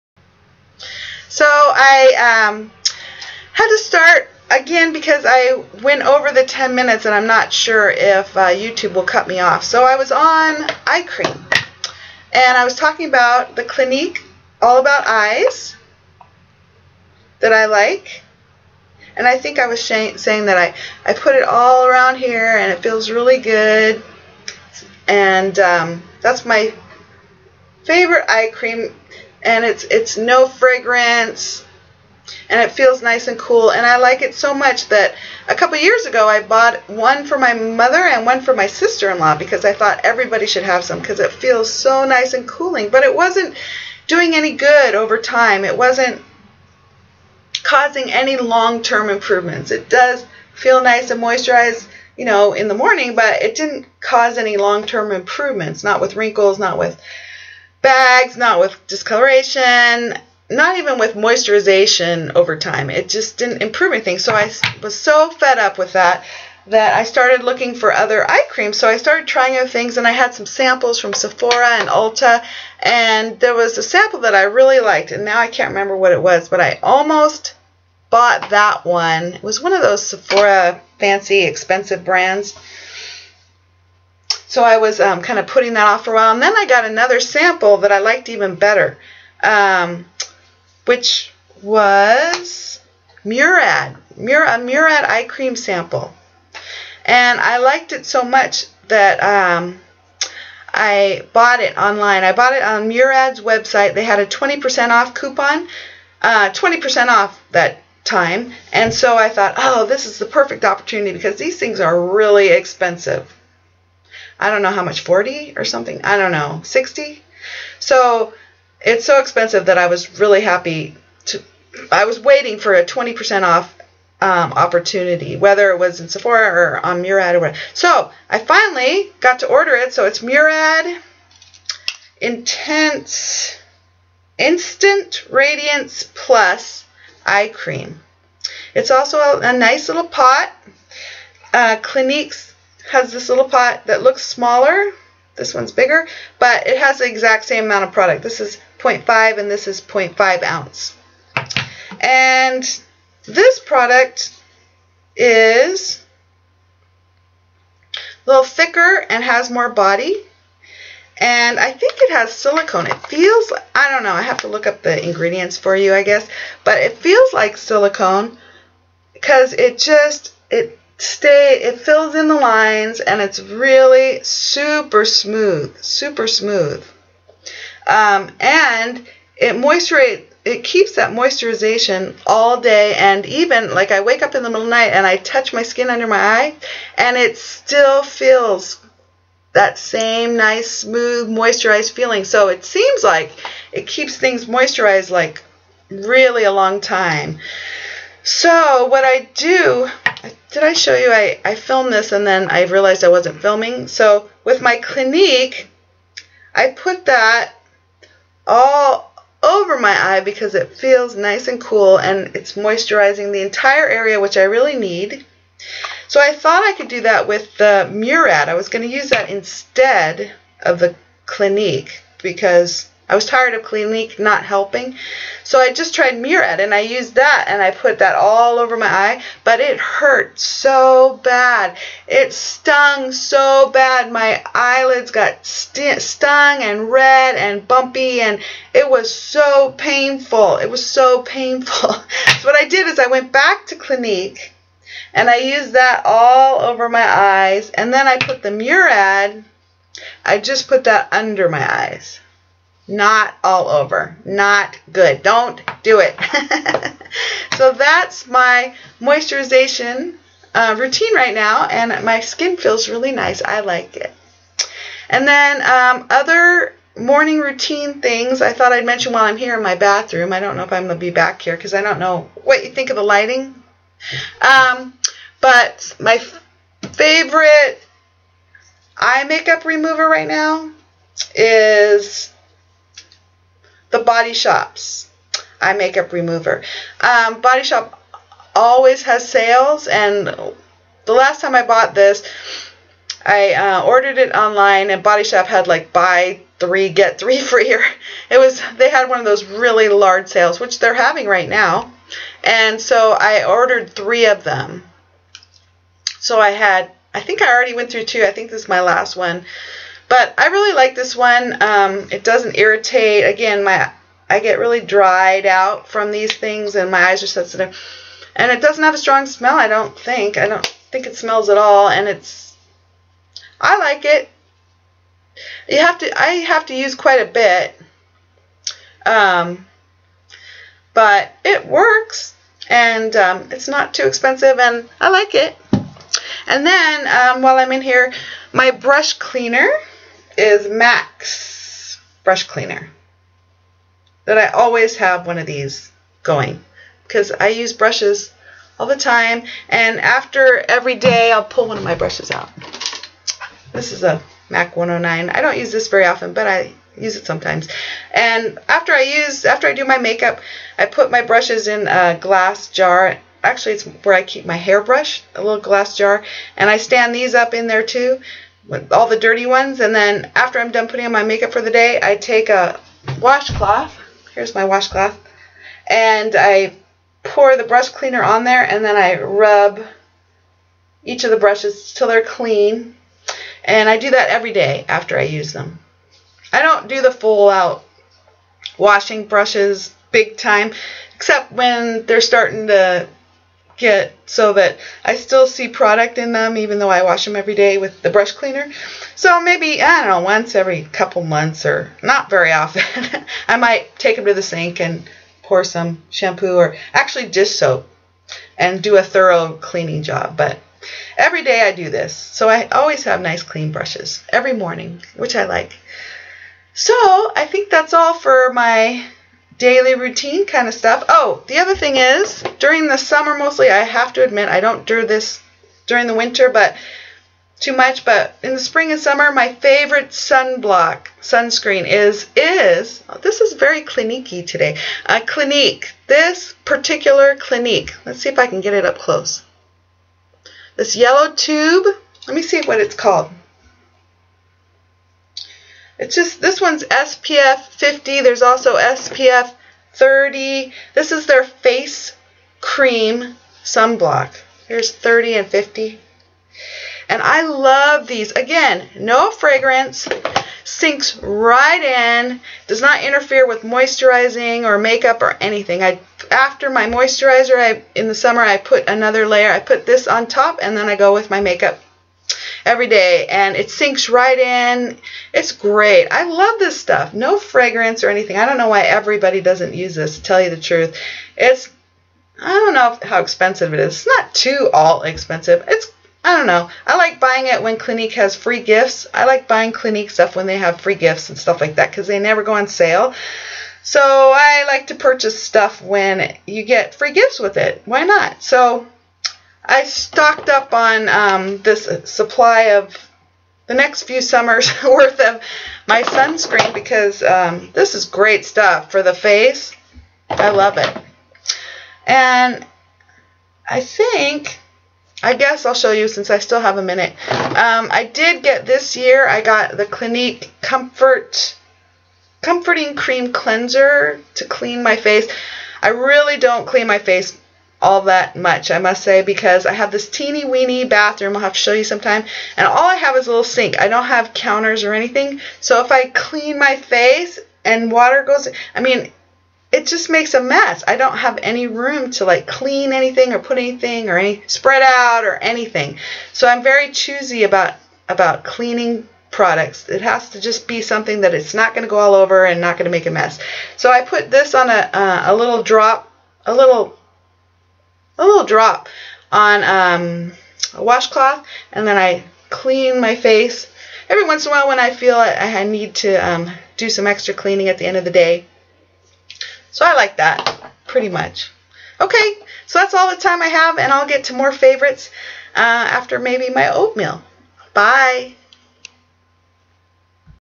Favorite Morning Routine Dec 2013, Part 2 - Skin, Eyes, Sunscreen, Eye Makeup Remover, Brush Cleaner
Hello! Part 2 of my current favorite morning routine, fresh out of the shower!  No makeup, again!  Hope you enjoy!

Products Mentioned:
Clinique All About Eyes
Murad Instant Radiance Eye Cream
Clinique Face Cream Sunscreen in 30 and 50
Clinique Comforting Cream Cleanser
The Body Shop Camomile Gentle Eye Make-Up Remover
MAC Brush Cleanser

I paid for all of these products, myself.  I do not get paid any money for any part of filming these videos!  This is my honest opinion.